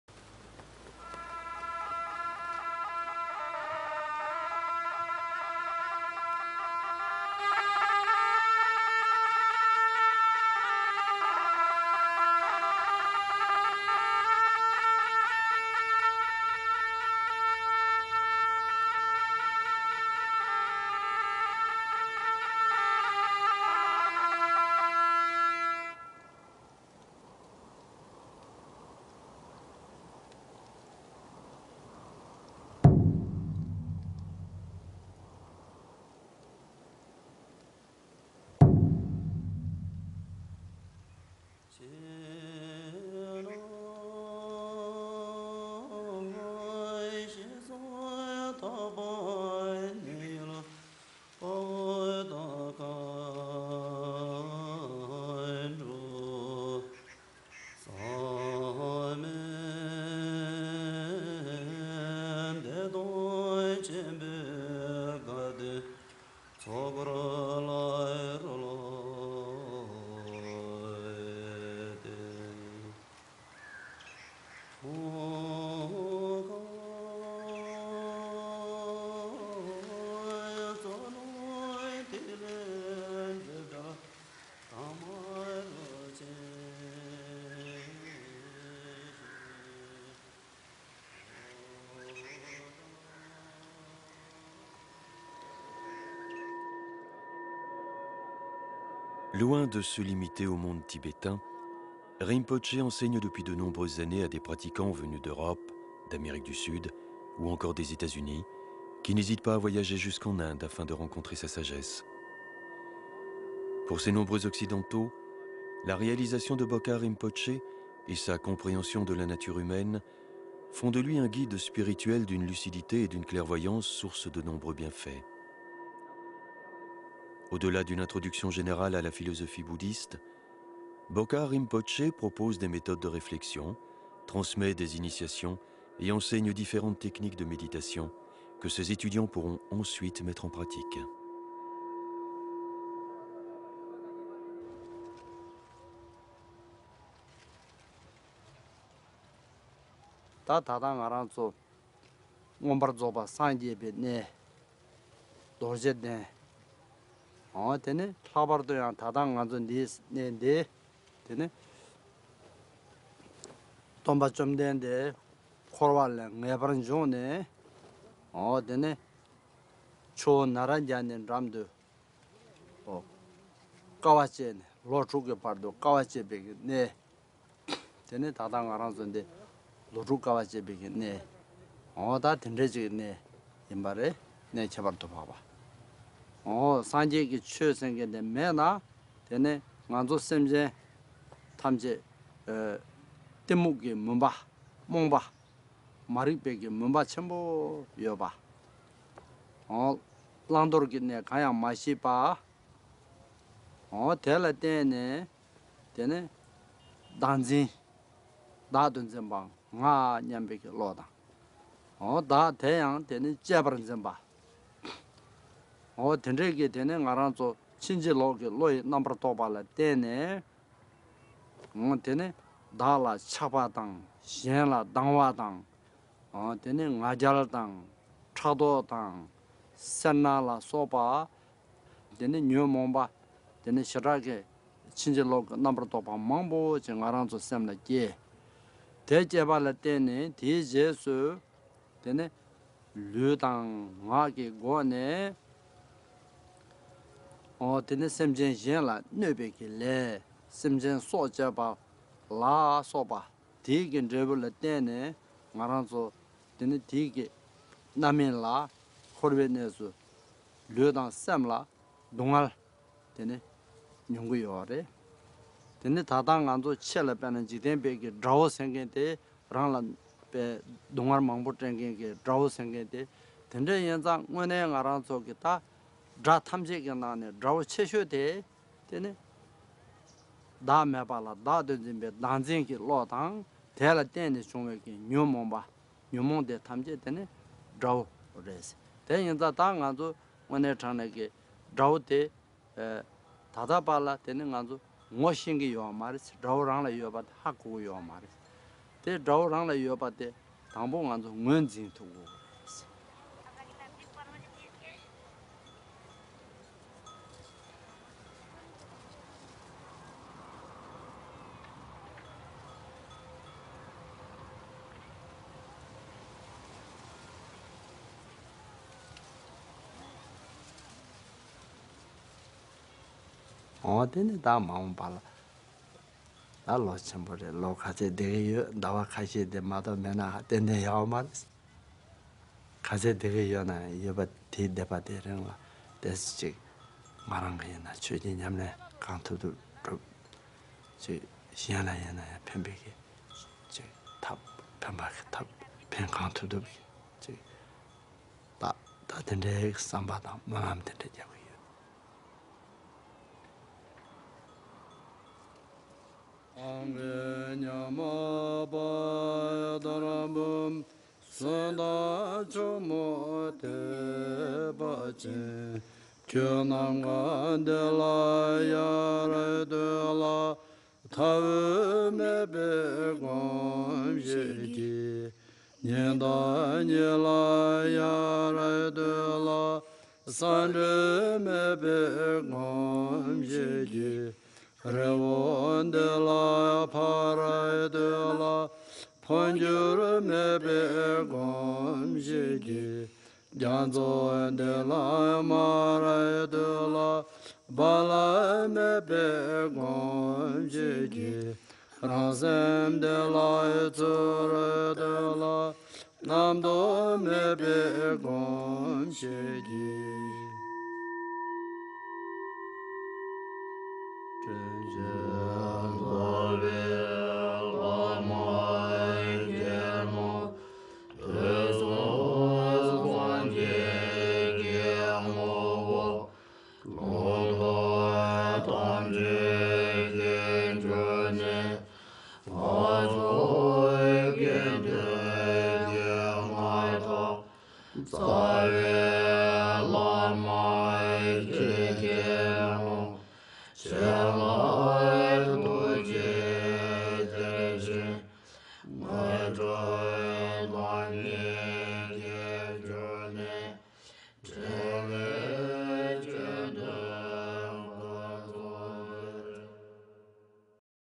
Bodhisattva
Explication des "voeux de Bodhisattva" par le Très Vénérable Kyabje Bokar Rinpoche

(extrait du film de Guy Maezelle)